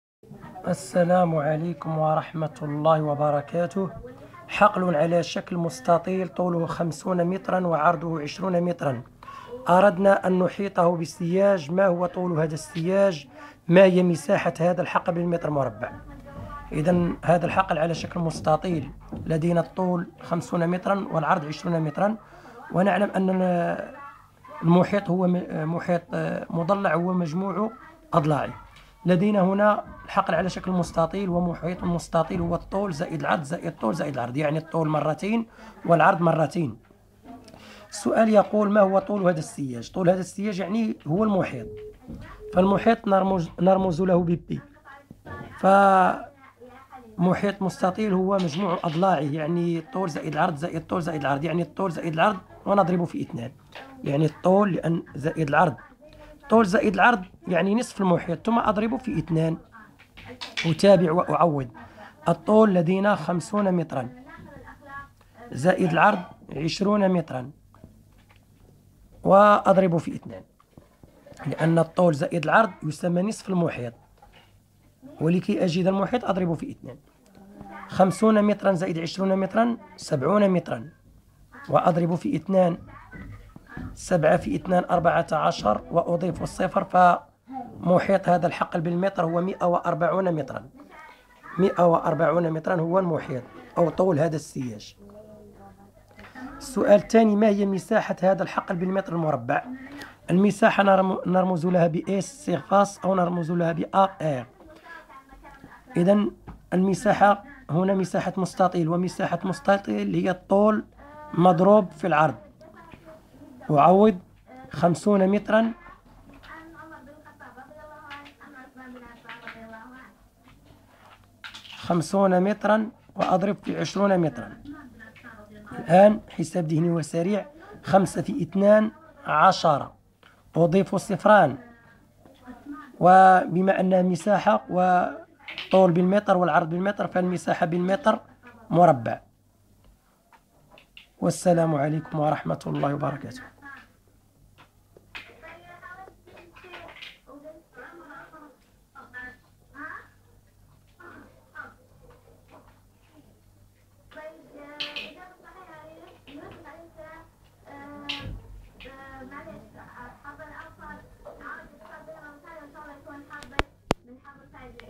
حساب محيط ومساحة المستطيل
محيط المستطيل يساوي الطول زائد العرض ثم اضرب الكل في اثنان ومساحة المستطيل هي الطول في العرض والله المستعان